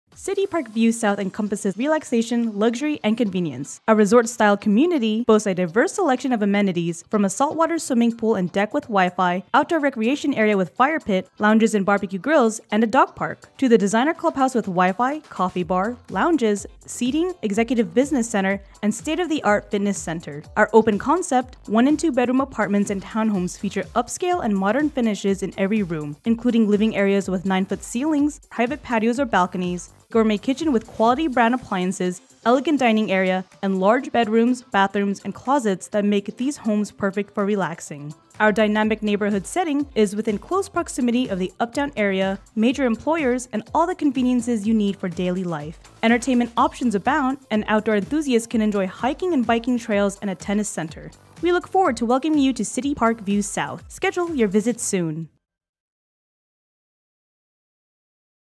CityPark View South Apartments in Charlotte, NC - ForRent.com
CityPark View South Apartments for rent in Charlotte, NC on ForRent.com: Availability, pricing and special promotions subject to change daily. Discover new heights in apartment home living at CityPark View South Apartments in Charlotte, North Carolina. With new one and two bedroom apartments and townhomes that incorporate modern designer finishes and resort-style amenities in the heart of the historic Charlotte Coliseum district - home to many of the areas major employers. Apartment homes include open-concept living with gourmet-style kitchens and elegant dining areas, upscale finishes throughout, and private patios where you can relax and unwind. Indulge yourself with PAC Concierge services and 24-hour community amenities such as a tech-inspired, state-of-the-art fitness center, a Starbucks coffee bar, and convenient business center. Other amenities include a sparkling salt-water swimming pool and large poolside deck with complimentary Wifi access. Situated in a unique location, CityPark View South Apartments provides access to a dynamic and vibrant neighborhood and popular destinations like the Charlotte Regional Farmers Market, Uptown Charlotte, South Park, South End, and more.  So call CityPark View South Apartments today to schedule a private tour.

 - 1710 Piedmont Hills Place